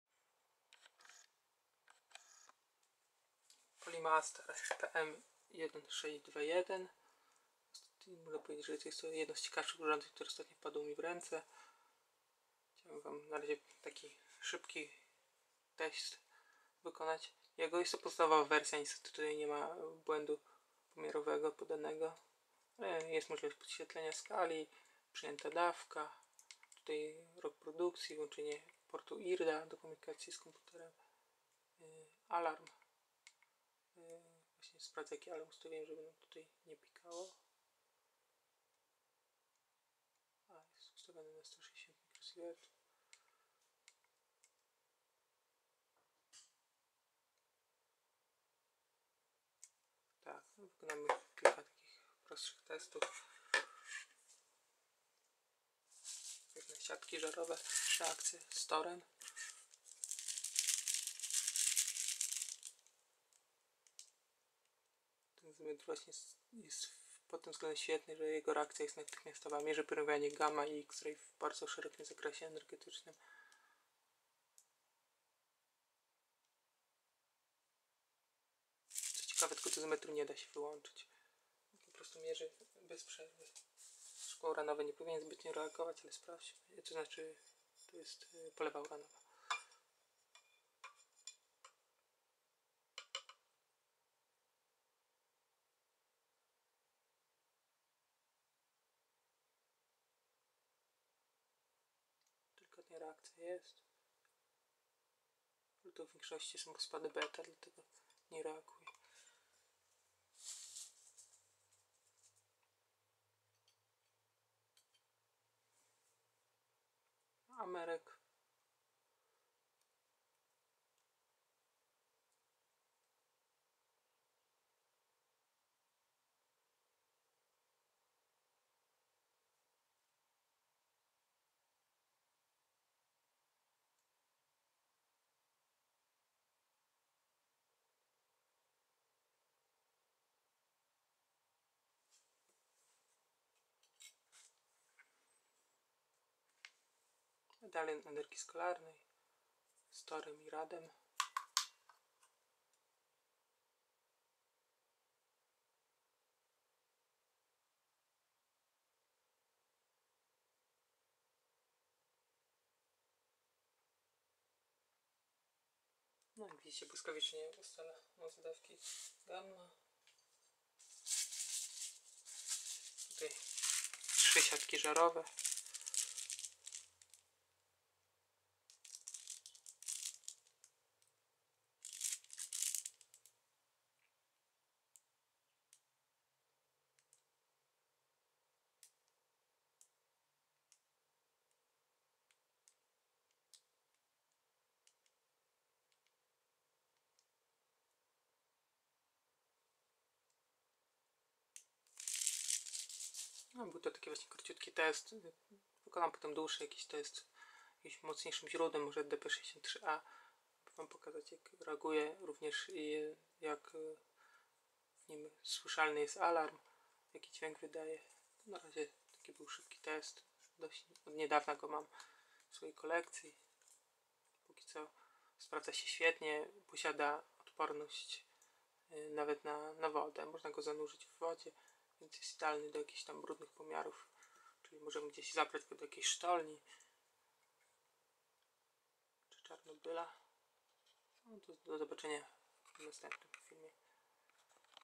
Dosimeter Dozymetr Polimaster PM1621 Quick Test (X-Ray/Gamma)
Krótki test dozymetru Polimaster PM1621, pełna i szczegółowa recenzja na blogu